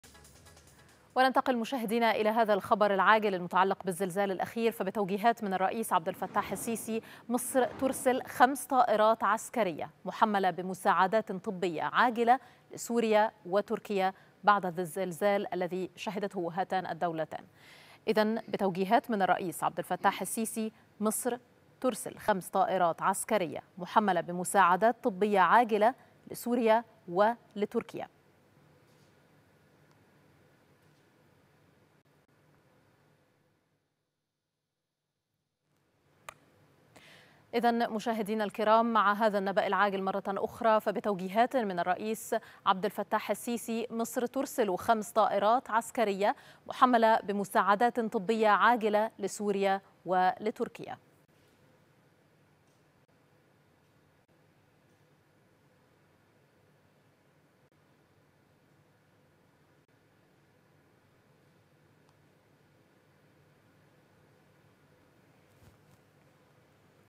بتوجيهات من الرئيس السيسي.. مصر ترسل 5 طائرات عسكرية محملة بمساعدات طبية عاجلة لسوريا وتركيا
مال وأعمال| بتوجيهات من الرئيس السيسي.. مصر ترسل 5 طائرات عسكرية محملة بمساعدات طبية عاجلة لسوريا وتركيا

7 فبراير 2023